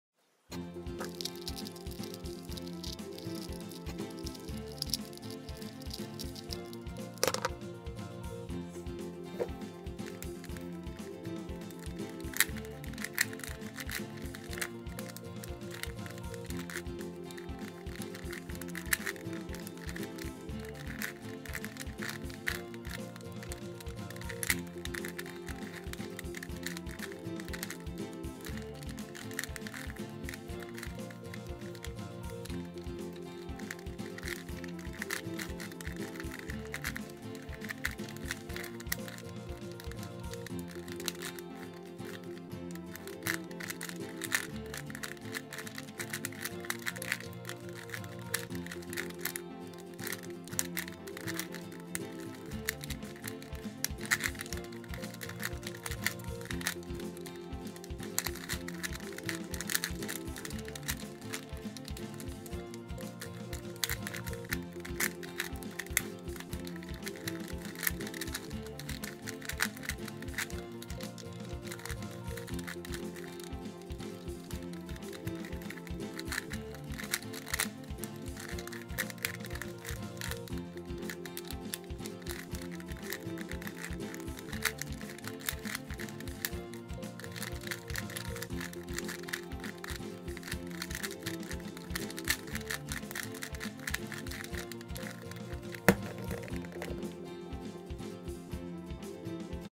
Megamorphix Solve
-The video is sped up by 50%
-The solve took about 2 mins and 20 seconds